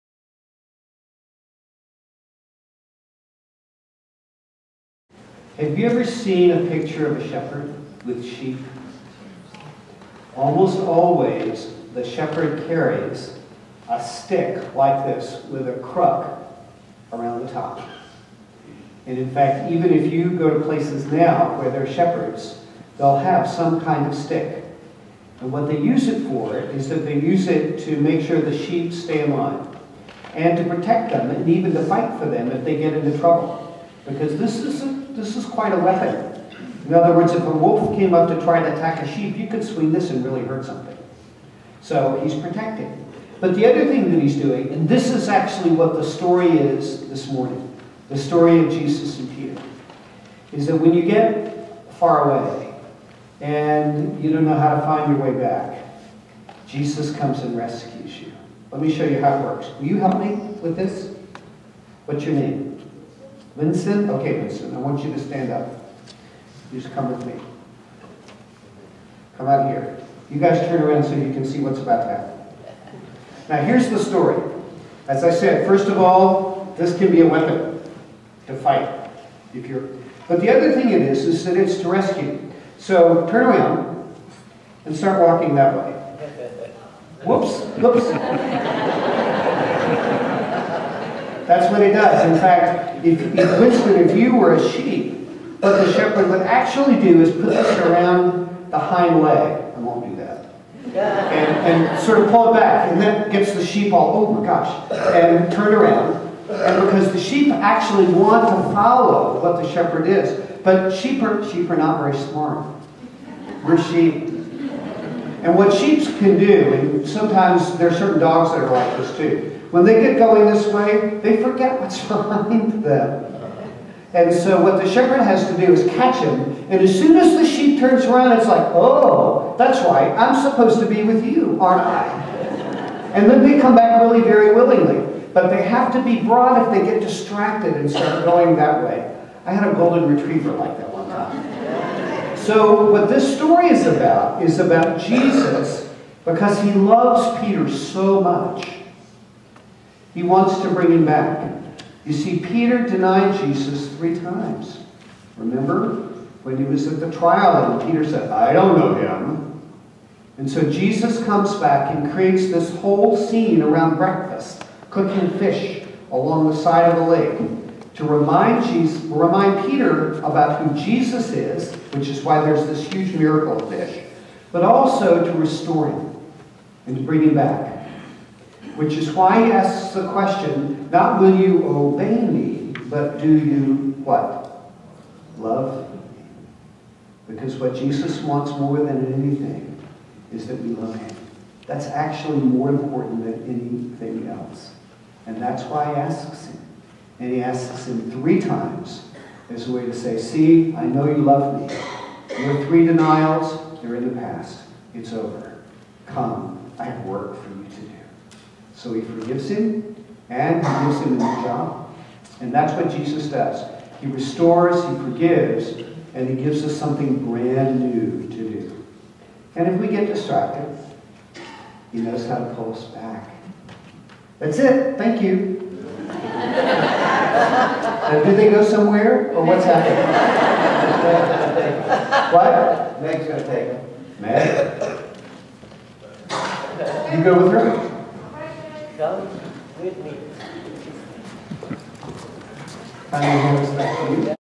Bishop Brewer explains the Crozier
Bishop Gregory Brewer, Episcopal Diocese of Central Florida, tells kids at St. Mary's Episcopal Church, Belleview, about the Bishop's Crozier. April 14, 2013.